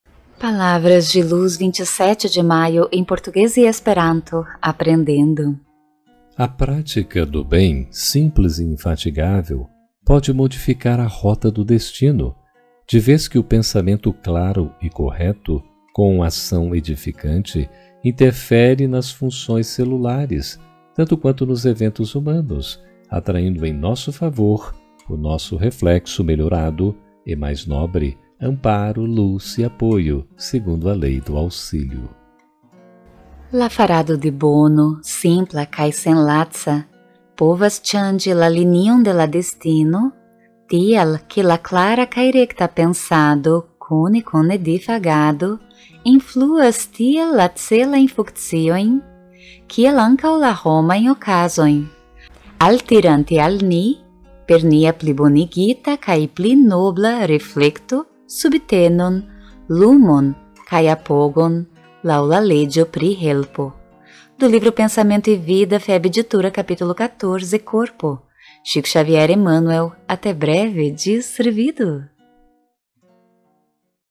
Palavras de Luz em Português e Esperanto - 27 de maio de 2024
Mensagem do Dia
Livro Palavras de Luz
FEB Editora
Em Português e Esperanto.
Nas vozes de Elaine Kapp e Geraldo Campetti.
Tradução Affonso Soares.
Você pode consultar também pelos canais do Facebook e Instagram da @feb_oficial e @febeditora.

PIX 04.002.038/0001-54

Gratidão!